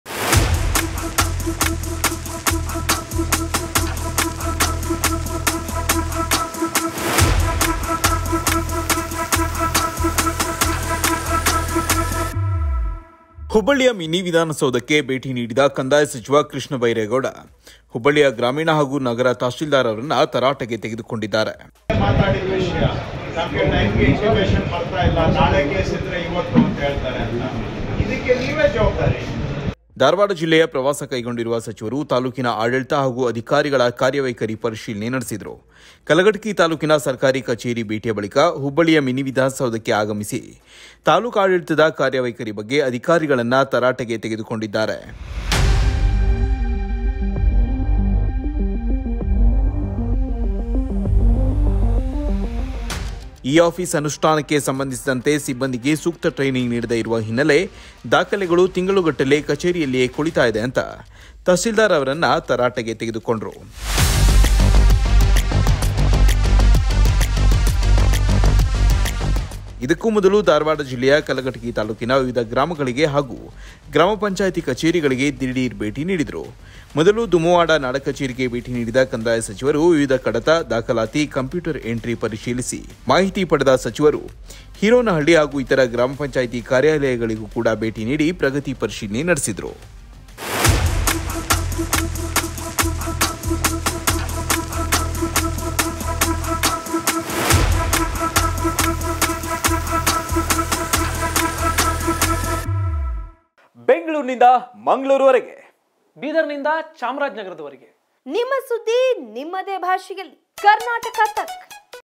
Krishna Byre Gowda: ಸಚಿವ ಕೃಷ್ಣಭೈರೇಗೌಡ ಕಚೇರಿ ರೌಂಡ್ಸ್, ಅಧಿಕಾರಿಗಳಿಗೆ ಸಖತ್ ಕ್ಲಾಸ್ | Hubli News
Minister Krishna Byre Gowda, who is on a tour of the Dharwad district, lashed out at the Taluk Officials in Hubbali over their administration.

ಹುಬ್ಬಳ್ಳಿಯ ಮಿನಿವಿಧಾನಸೌಧಕ್ಕೆ ಭೇಟಿ ನೀಡಿದ ಕಂದಾಯ ಸಚಿವ ಕೃಷ್ಣ ಭೈರೇಗೌಡ ಅವರು ಹುಬ್ಬಳ್ಳಿಯ ಗ್ರಾಮೀಣ ಹಾಗೂ ನಗರ ತಹಶಿಲ್ದಾರರರನ್ನು ತರಾಟೆಗೆ ತೆಗೆದುಕೊಂಡಿದ್ದಾರೆ.
ಧಾರವಾಡ ಜಿಲ್ಲೆಯ ಪ್ರವಾಸ ಕೈಗೊಂಡಿರುವ ಸಚಿವರು ತಾಲೂಕಿನ ಆಡಳಿತ ಹಾಗೂ ಅಧಿಕಾರಿಗಳ ಕಾರ್ಯವೈಖರಿ ಪರಿಶೀಲನೆ ನಡೆಸಿದ್ರು. ಕಲಘಟಗಿ ತಾಲೂಕಿನ ಸರ್ಕಾರಿ ಕಚೇರಿಯ ಭೇಟಿಯ ಬಳಿಕ ಹುಬ್ಬಳ್ಳಿಯ ಮಿನಿವಿಧಾನಸೌಧಕ್ಕೆ ಆಗಮಿಸಿ ತಾಲೂಕಾಡಳಿತದ ಕಾರ್ಯವೈಖರಿ ಬಗ್ಗೆ ಅಧಿಕಾರಿಗಳನ್ನು ತರಾಟೆಗೆ ತೆಗೆದುಕೊಂಡಿದ್ದಾರೆ.